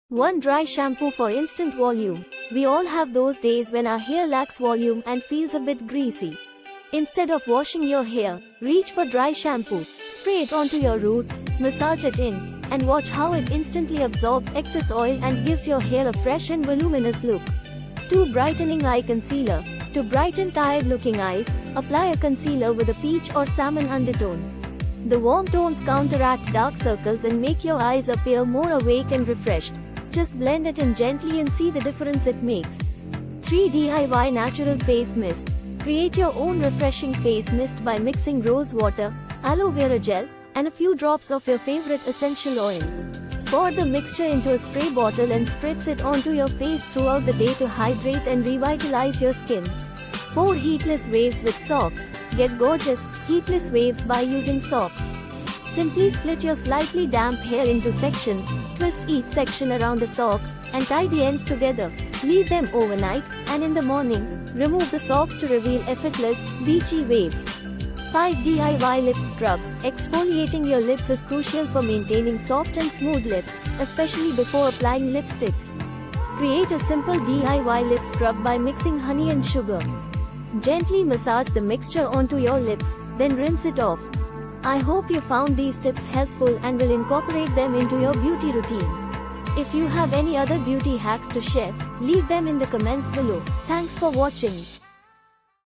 Revealing 5 Essential Beauty Hacks Every Girl Should Know
To enhance your beauty effortlessly, remember these five essential hacks: moisturize daily, use a primer for long-lasting makeup, exfoliate regularly, apply lip balm before lipstick, and prioritize quality sleep.glow up#glow up tips#hygiene tips#beauty tips#trending#skincare routines#how to#how to glow up